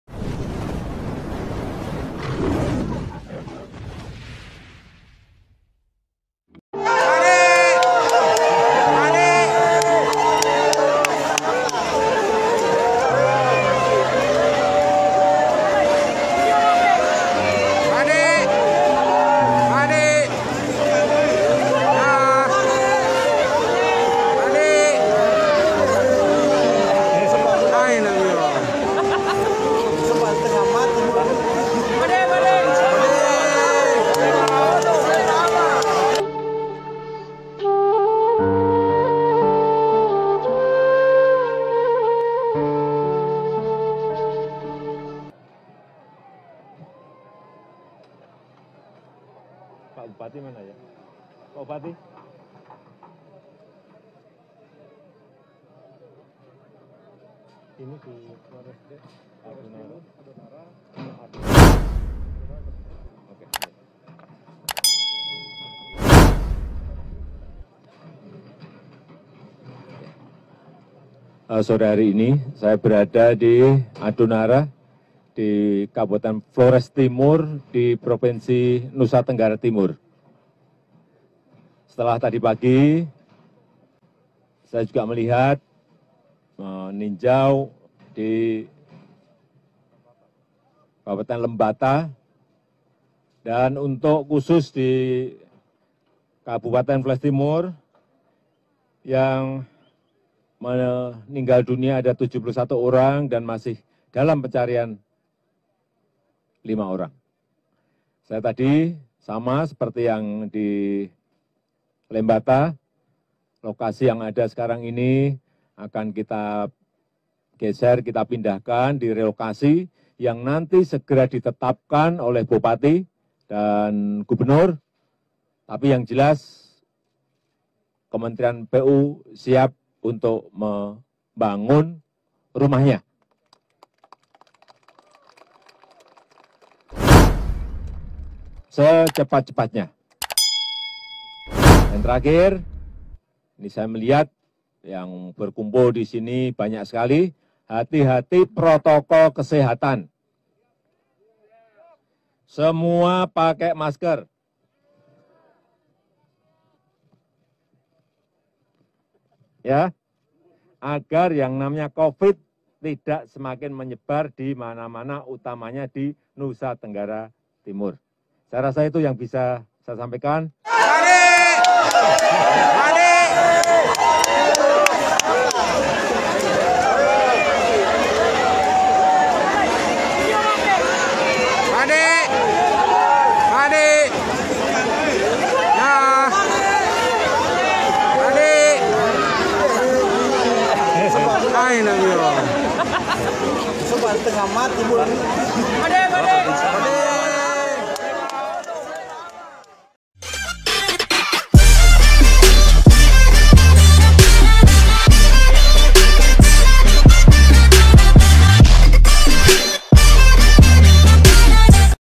Detik2 Air Mata Sang Presiden Tumpah Di Pulau Adonara
Air Mata Jokowi Tumpah di Adonara, Secepatnya Pemukiman Warga Dibangun.
Duka yang dialami warga Adonara, Kabupaten Flores Timur, Provinsi NTT dirasakan juga oleh Presiden RI, Joko Widodo. Ketika mengunjungi Desa Nele Lamadike, Kecamatan Ile Boleng, Pulau Adonara, Kabupaten Flores Timur, Jumat (9/4/2021), Presiden Jokowi meneteskan air mata. Rupanya Kepala Negara tak kuasa menahan haru dan ikut merasakan duka yang menelan 71 jiwa di Adonara, Minggu (4/4/2021) silam.
Presiden menangis ketika turun dari mobil kepresidenan dan disambut histeris warga Pulau Adonara yang telah menantinya sepanjang hari. Kepala Negara melambaikan tangan kepada semua warga yang tengah dirundung duka. Bahkan, Kepala Negara beberapa kali meneteskan air mata. “Bapak Presiden menangis ketika disambut oleh masyarakat di Nele Lamadike. Beliau mengusap air matanya beberapa kali,” sebut warga Adonara Barat yang menyaksikan langsung kunjungan Presiden di Nele Lamadike, Kecamatan Ile Boleng, Kabupaten Flores Timur.
Usai meninjau lokasi bencana di Adonara, Kepala Negara mengatakan, ketika melihat di lapangan memang keadaannya bebatuan, batu yang besar, yang itu sangat menyulitkan juga alat-alat berat. “Tetapi tadi sudah saya perintahkan agar terus dicari dan ditemukan yang masih hilang,” sebut Presiden Jokowi.
Presiden Jokowi juga berjanji, masyarakat di Adonara yang tertimpa musibah akan segera direlokasi. Tentang lokasi relokasi, Kepala Negara meminta Gubernur NTT, Viktor B Laiskodat dan Bupati Flotim Anton Hadjon untuk segera menentukan lokasi agar kementrian PUPR segera membangun rumah bagi korban bencana. “Yang jelas, kementerian PUPR akan siap untuk membangun rumahnya, secepatnya,” ujarnya.
Di Nele Lamadike, Kepala Negara juga mengingatkan warga di lokasi bencana untuk tetap menerapkan protokol kesehatan. “Saya melihat yang berkumpul di sini banyak sekali, hati-hati protokol kesehatan semua pakai masker, ya, agar yang namanya Covid-19 tidak semakin menyebar di mana-mana utamanya di Nusa Tenggara Timur,” ujar Presiden Jokowi